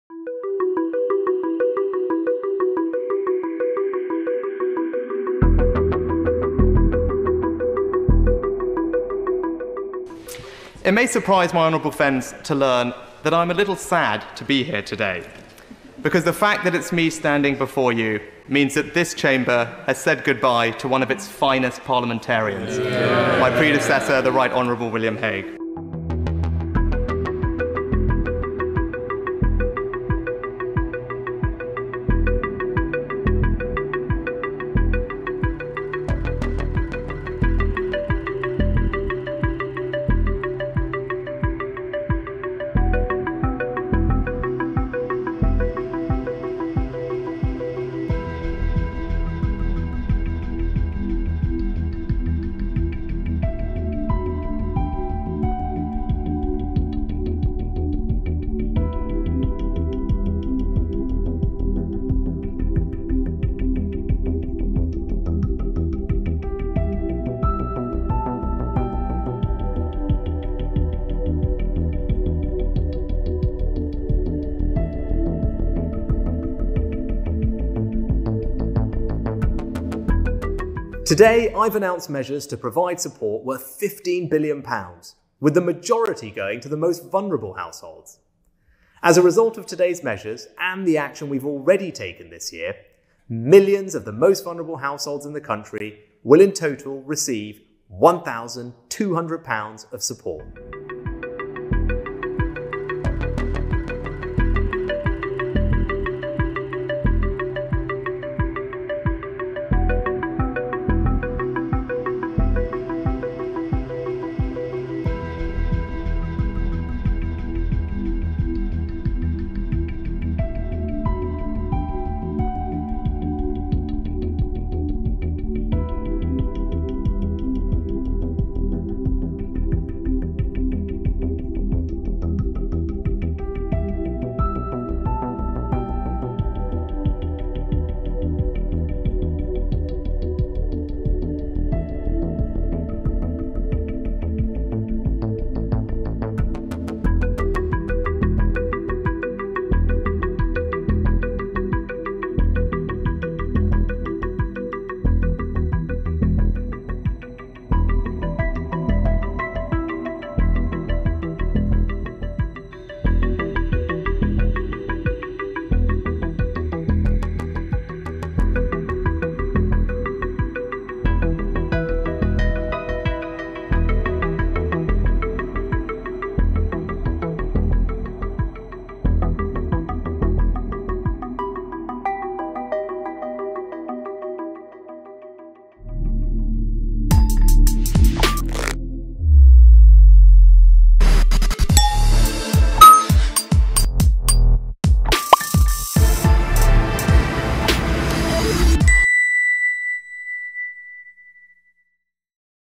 Who is Rishi Sunak, the Indian-Origin MP Who's in the Race to be New UK PM?
Chancellor of the Exchequer of the United Kingdom RishiSunak resigned from his post on Tuesday, 5 July, citing Prime Minister BorisJohnson's leadership. Sunak was and continues to be one of the favourites to be Johnson's successor, which, if it happens, would make him the first Indian-origin prime minister of the UK.